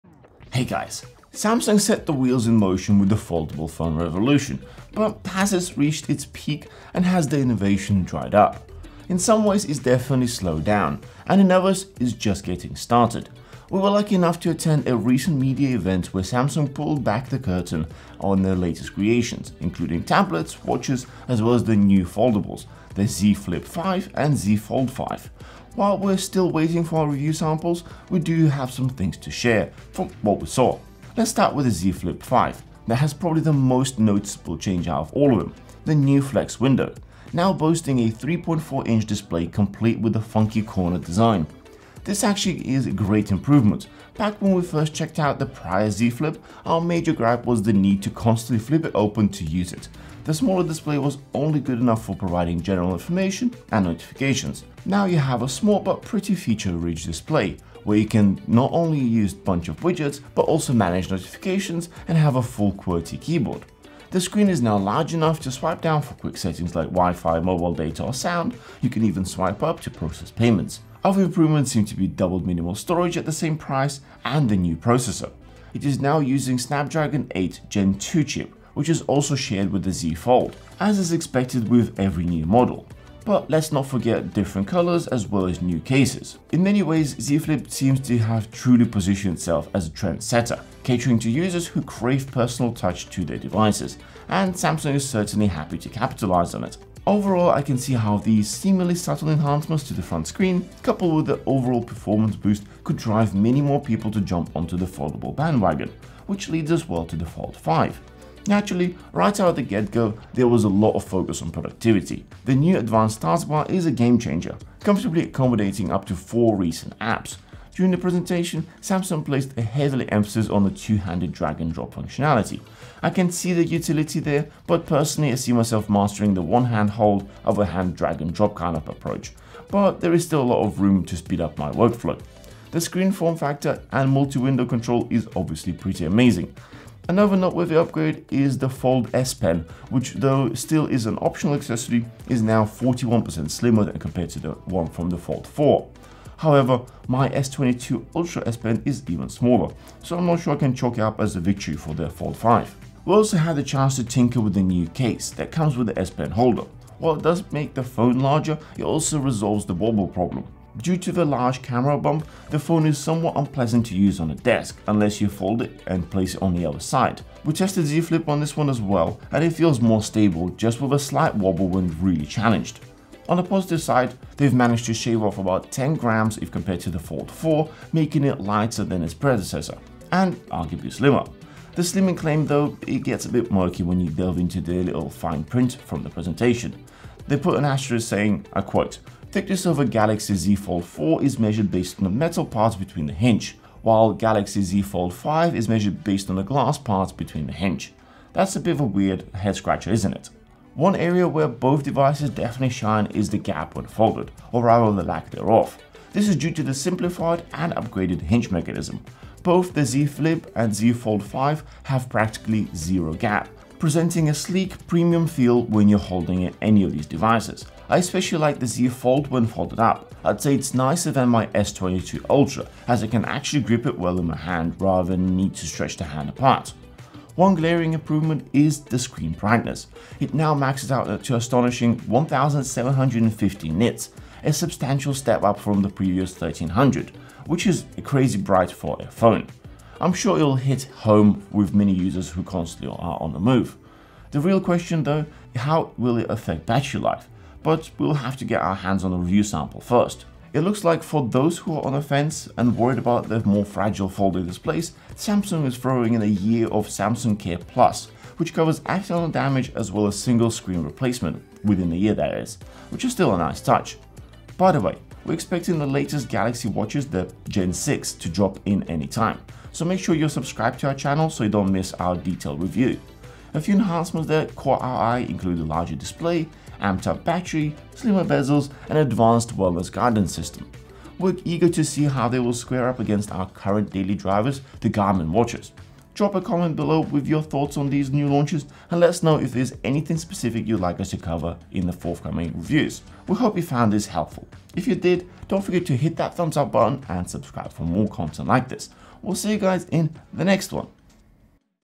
New Samsung Flipping good or Flopping bad // Z Flip 5 and Z Fold 5
Samsung set the wheels in motion with the foldable phone revolution, but has it reached its peak and has the innovation dried up? In some ways it has definitely slowed down and in others it is just getting started. We were lucky enough to attend a recent media event where Samsung pulled the curtain back on their latest creations, including tablets, watches, as well as the new foldables - the Z Flip5 and Z Fold5. While we are still waiting for our review samples - we do have some things to share from what we saw.

Main Set:
Sit/Stand Ergonomic desk from Everdesk+
Supreme Chair from Ergotune

Our Gear

Main Camera

Chapters:
0:00 - Intro
0:31 - Z Flip5 Overview
1:48 - Z Flip5 Overview
4:25 - What's To Come